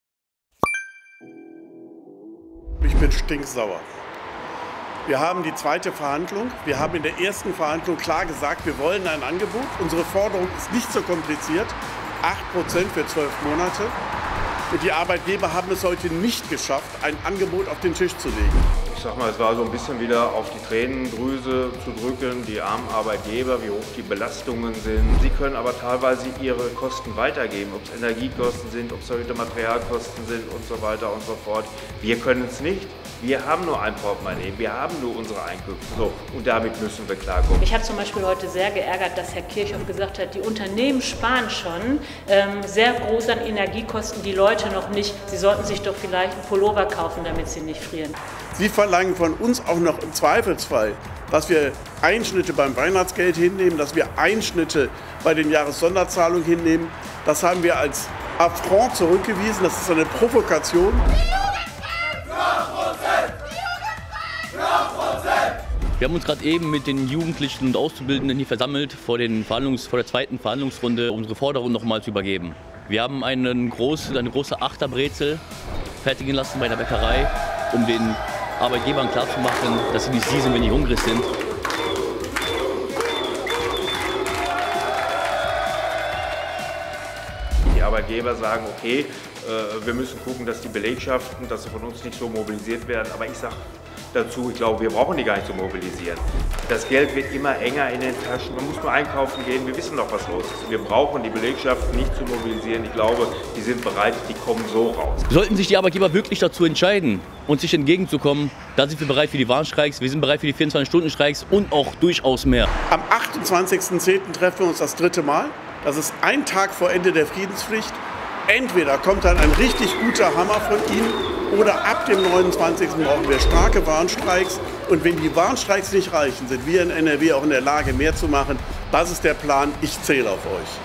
2. Verhandlung Metall und Elektro Tarifrunde
In der 2. Verhandlungsrunde gab es statt eines Angebotes von Arbeitgeberseite Forderungen. Was sie sich für diese Tarifrunde vorstellen und was die Arbeitnehmer davon halten, erfahrt ihr im Video.